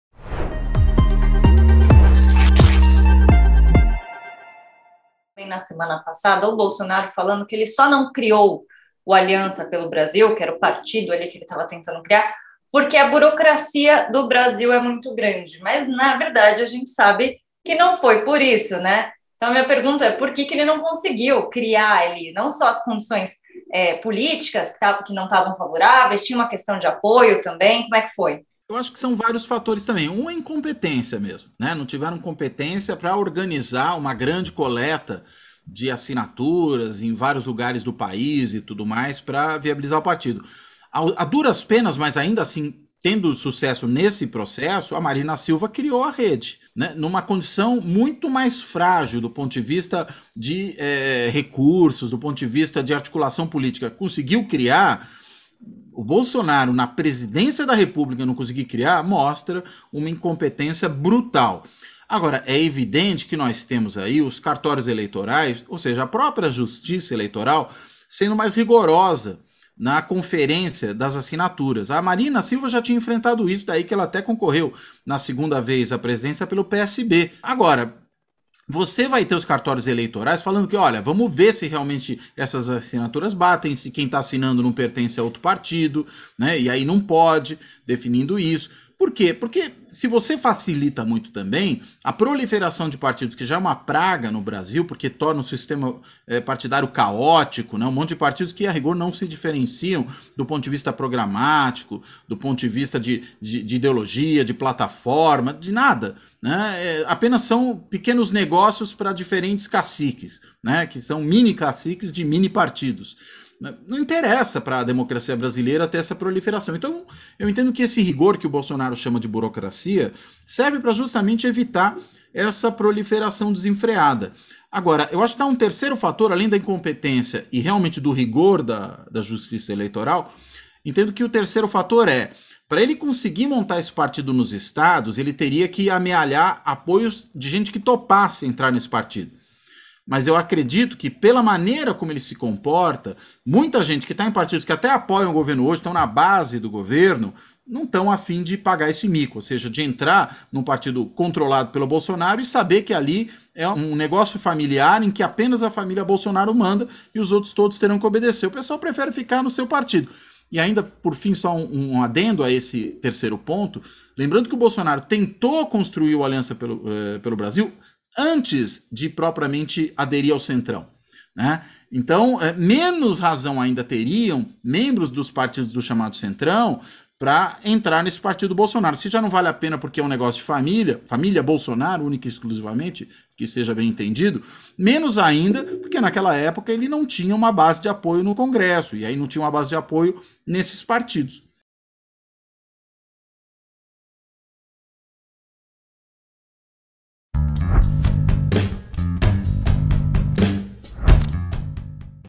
FALHA DA 'ALIANÇA PELO BRASIL' É RESULTADO DA INCOMPETÊNCIA DO BOLSONARISMO
Após uma semana de intensa expectativa, o presidente Jair Bolsonaro suspendeu sua filiação ao PL, o Partido Liberal de Valdemar Costa Neto, originalmente agendada para o dia 22/11. Nesta segunda-feira 15, o ex-capitão revelou que existe a possibilidade de ingressar em outra sigla do 'centrão'. Bolsonaro relatou, ainda, manter ainda conversas paralelas e que segue vivo o interesse do Progressistas e do Republicanos em filiá-lo. Por qual razão o presidente tem tanta dificuldade em encontrar um partido para disputar as eleições de 2022? Para responder essa questão, CartaCapital recebe o cientista político Cláudio Couto, da Fundação Getúlio Vargas.